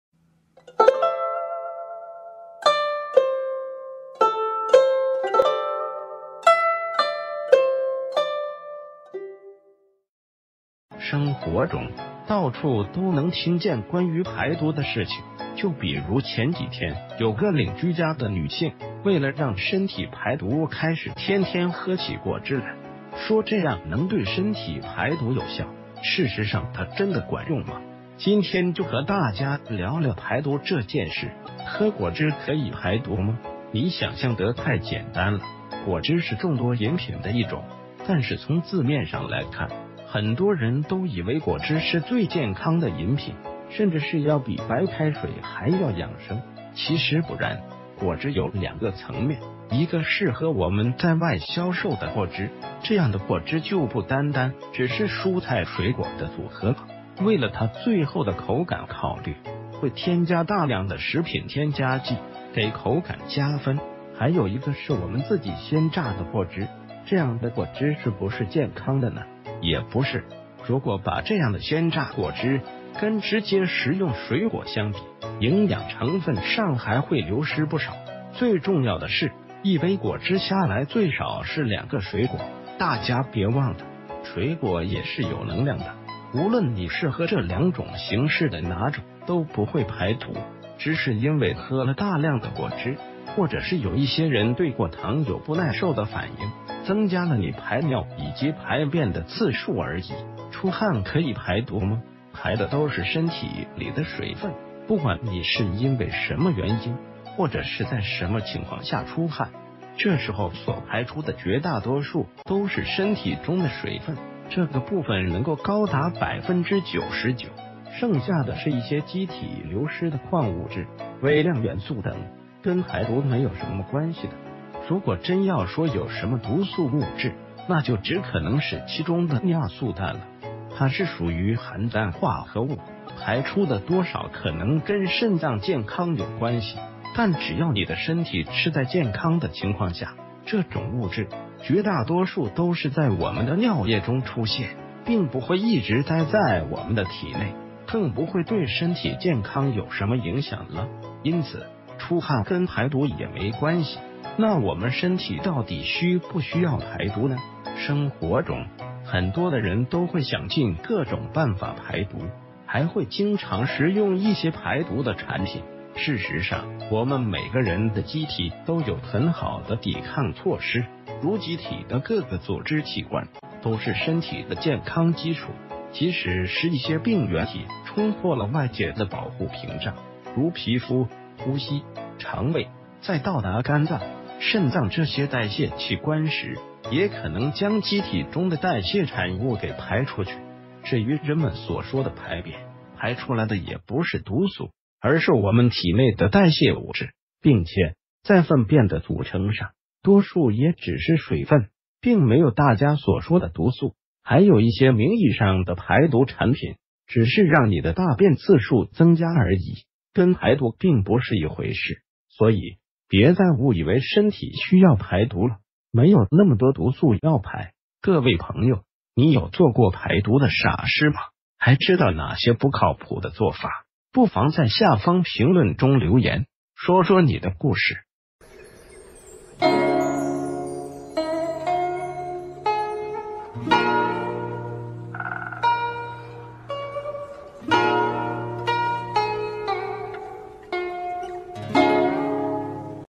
喝果汁可以排毒嗎？關于身體排毒的這3個真相，你該知道了
生活中，到處都能聽見...